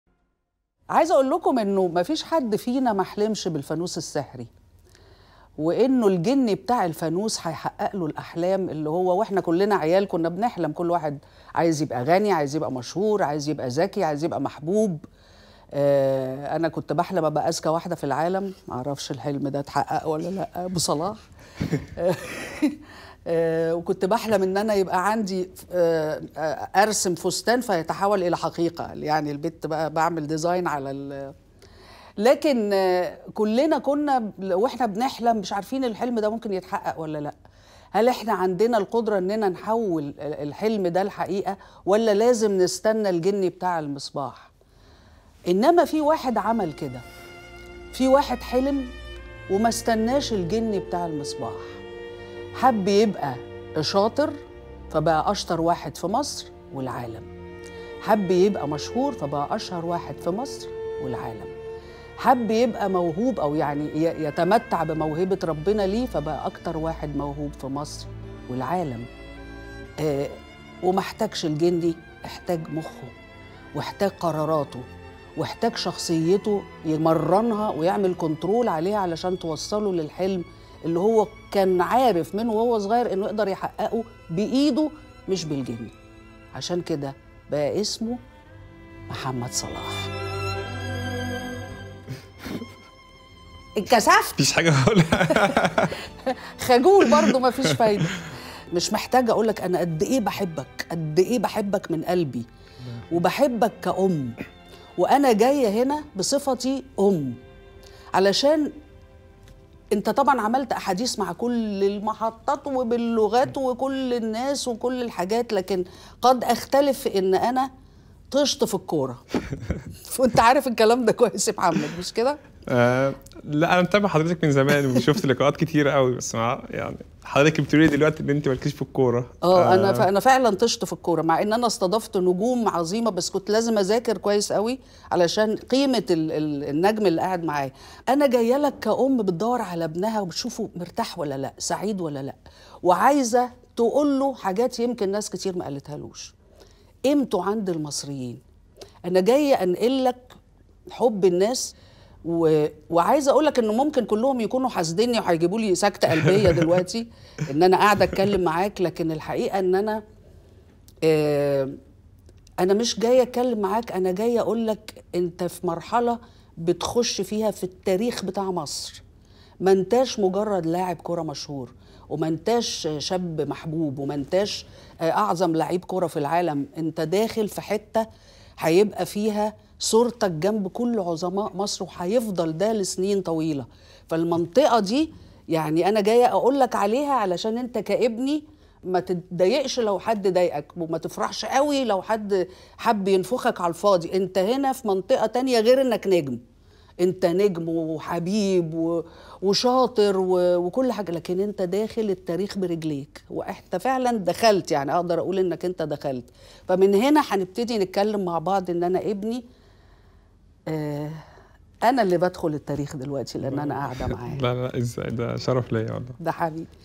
في واحد حلم ومستناش الجني بتاع المصباح لأنه عاش الحكاية وقرارته حكاية ألف ليلة وليلة لصلاح لسة مكملة
في واحد حلم وماستناش الجني بتاع المصباح، لأنه عاش الحكاية بعقله وقرارته حكاية ألف ليلة وليلة لصلاح لسة مكملة لليالي طويلة أجمل!